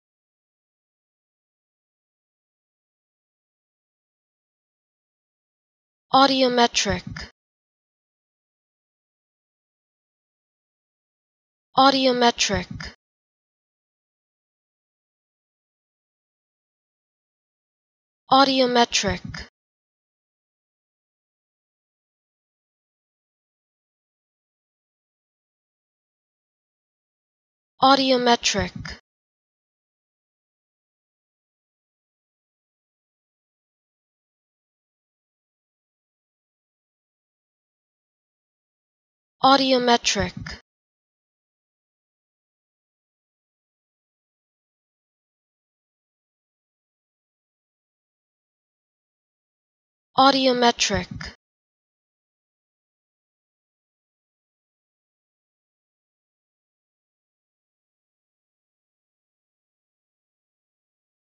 How To Say Audiometric
Learn how to say Audiometric with EmmaSaying free pronunciation tutorials.